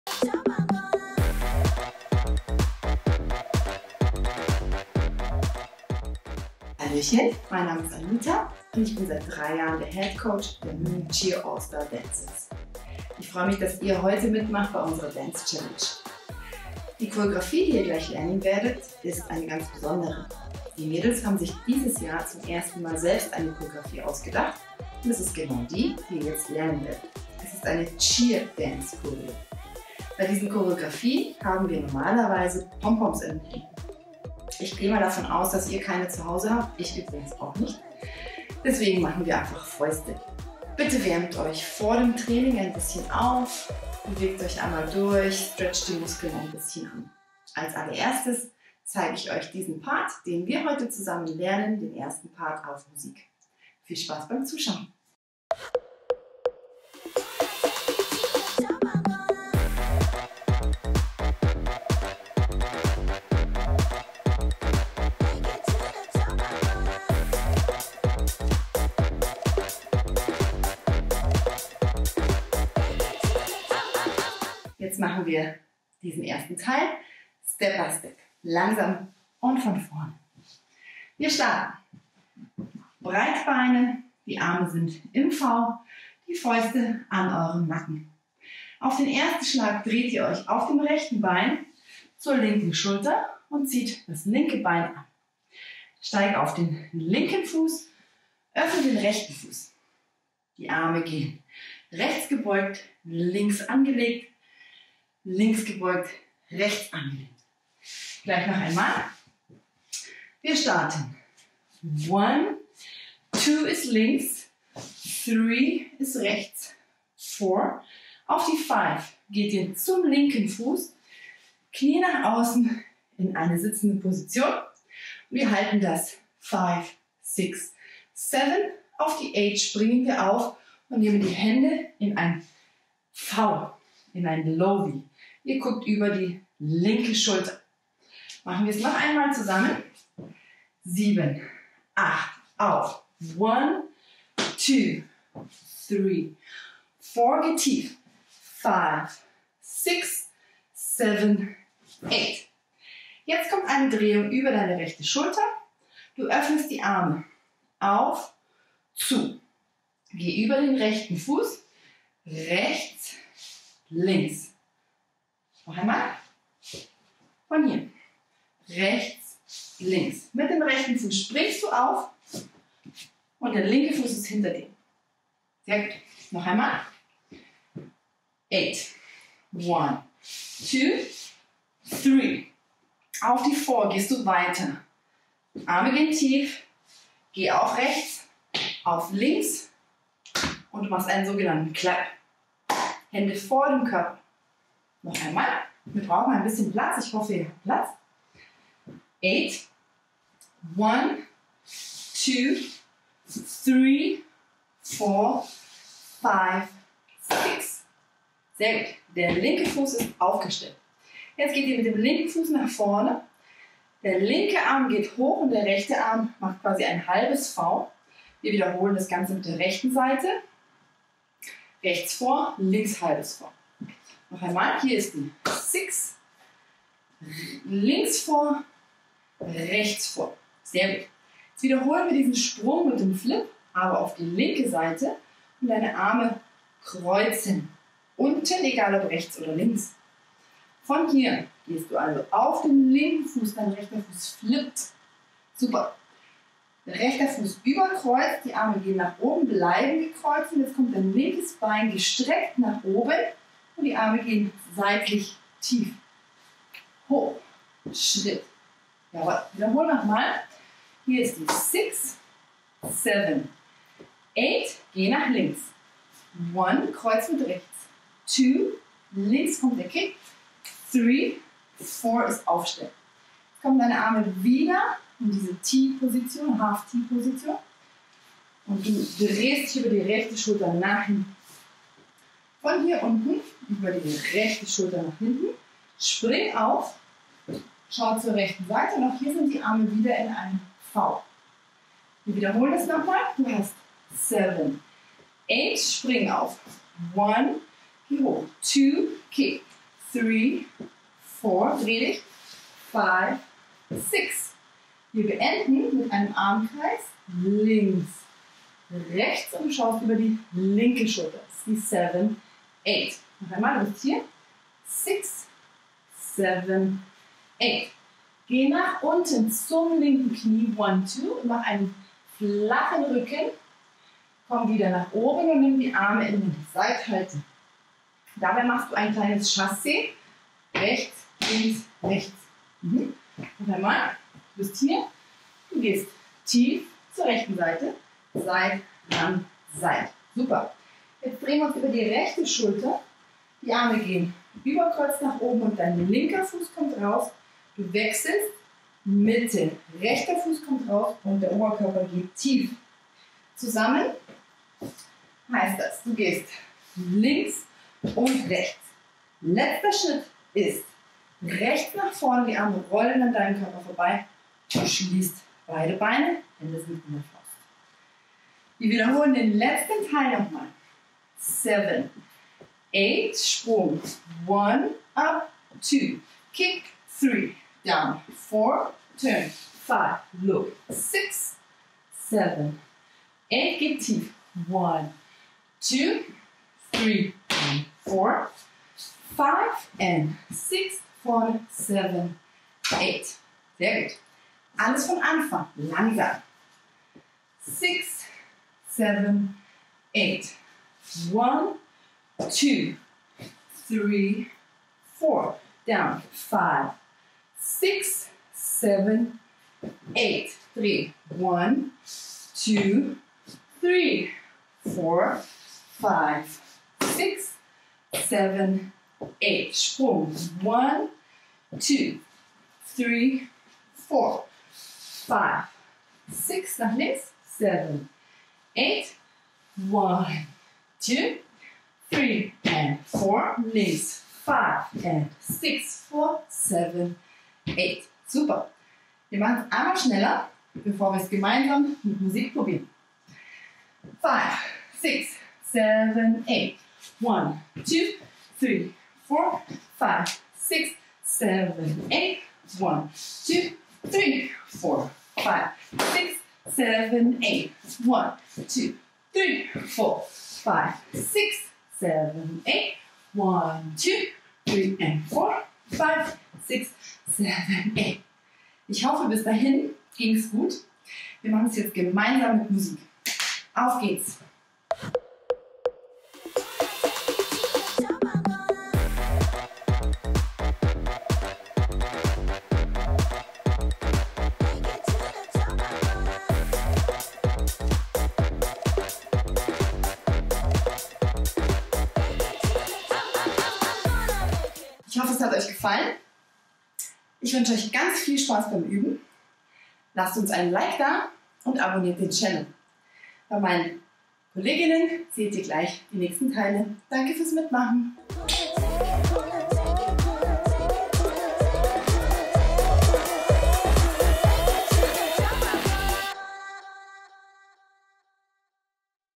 MCADANCECHALLENGE | Munich Cheer Allstars | PART 1/5
Seid dabei! Die Munich Cheer Allstars bringen euch in den nächsten 5 Tagen ihre selbst erstellte Choreographie bei. Heute PART 1 mit Headcoach Anita.

Morgen gehts dann mit PART 2 und Assistant Coach Sasha weiter.

Eure Tanz-Videos - sie müssen auf keinen Fall perfekt sein - bitte entweder mit dem @fcbayernbasketball verlinkt und dem Hashtag MCADANCECHALLENGE auf Instagram oder Facebook posten oder direkt an schicken.

Aus allen Videos erstellen wir am Ende eine große Choreographie, die dann bei uns auf dem Channel veröffentlicht wird. Macht mit!